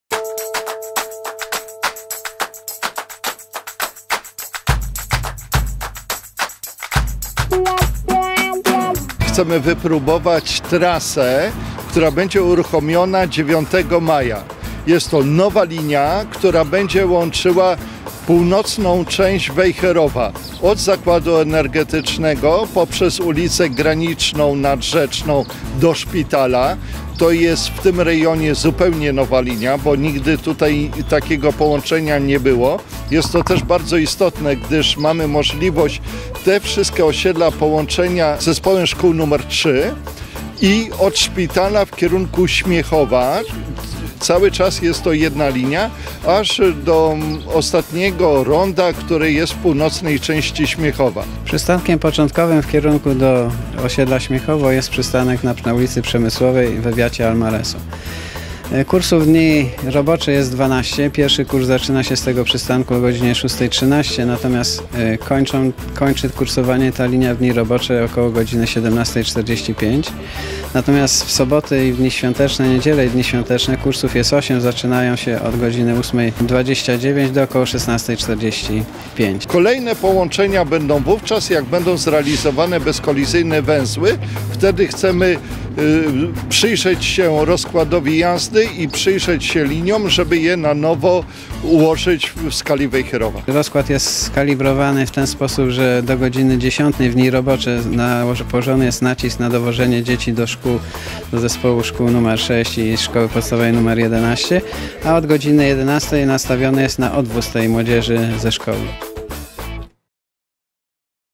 Wejherowo.pl - Nowa linia autobusowa nr 6
Nowa linia autobusowa nr 6, która połączy północną część Wejherowa – z rejonu Zakładu Energetycznego, ze Szpitalem Specjalistycznym i Śmiechowem, zostanie uruchomiona 9 maja br.